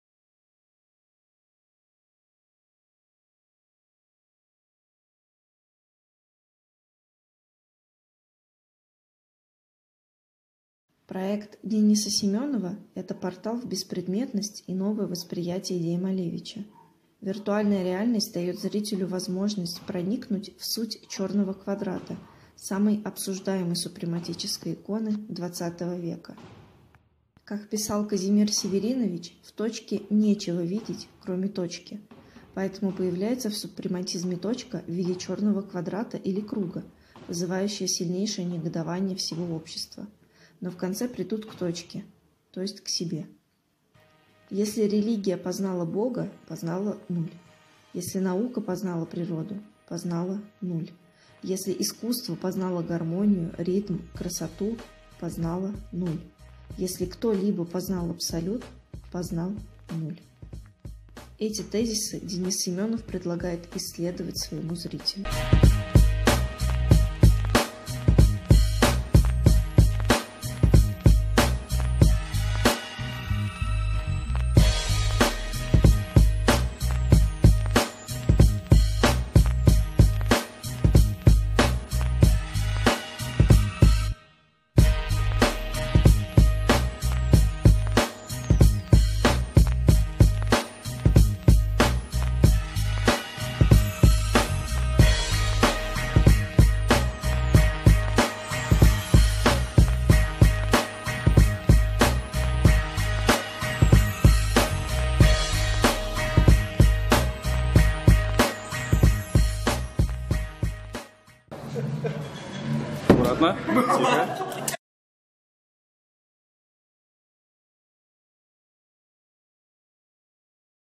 Денис Семёнов. Иммерсия 2.Nihil
Видео из цикла мини-экскурсий по проектам выставки "Открытый музей".

Адрес: г. Москва, ул. Ростокинская, 1